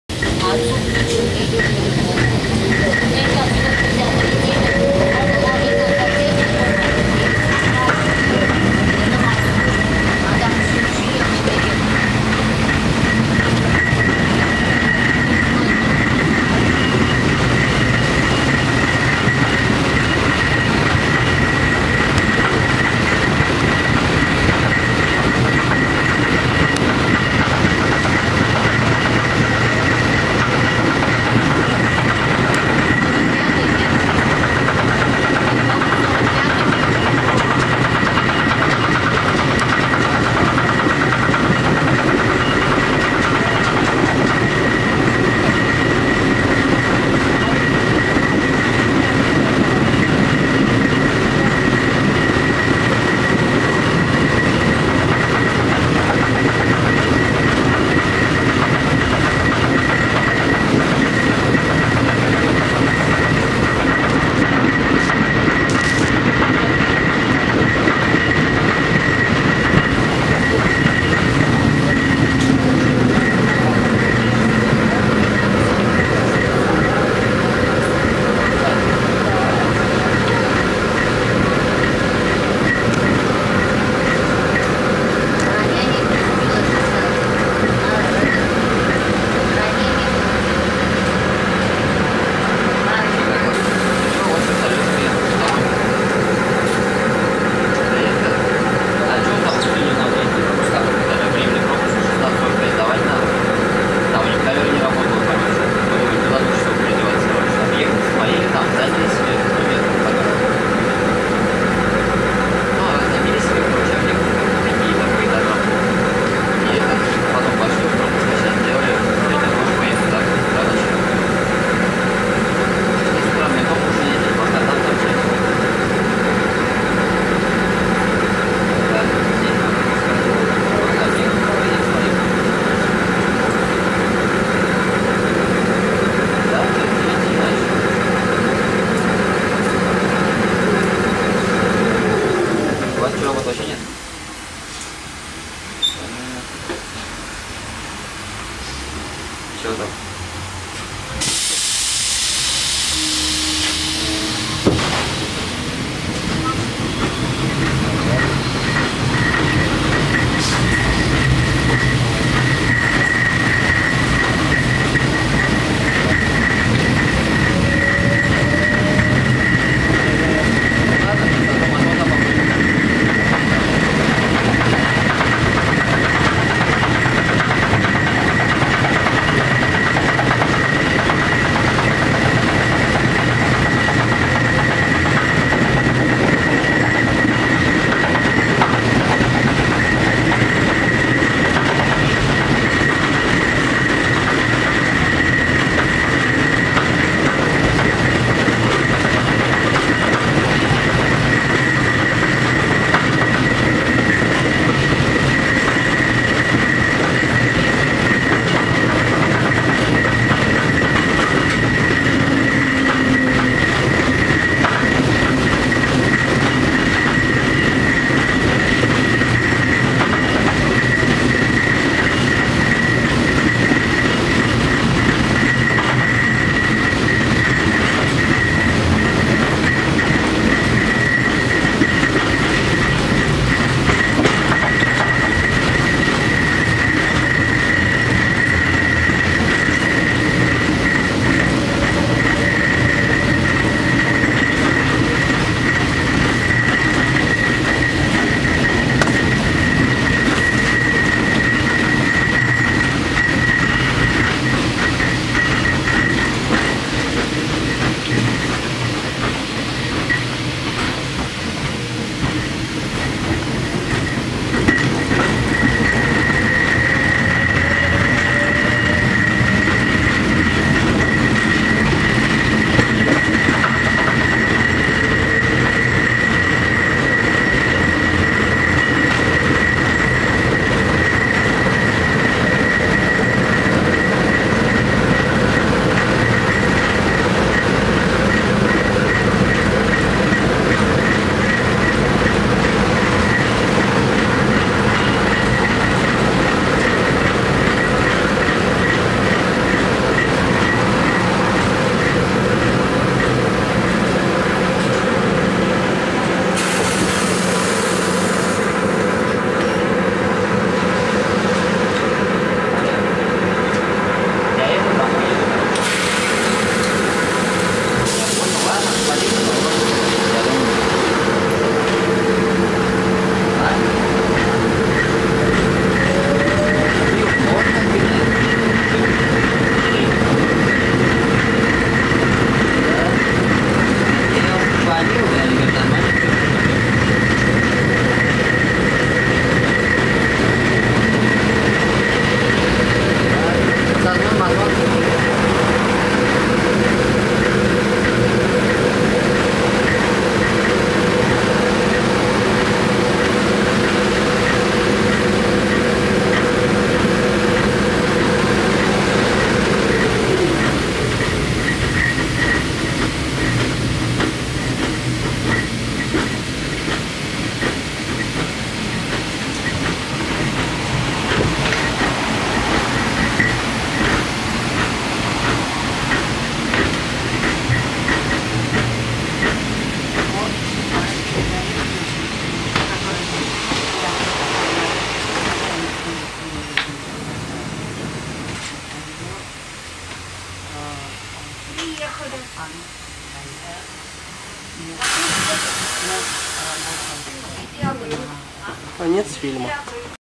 Казань, ЛМ-99К №1248, маршрут №1.
Участок Соцгород(м. Авиастроительная) - улица А.Королёва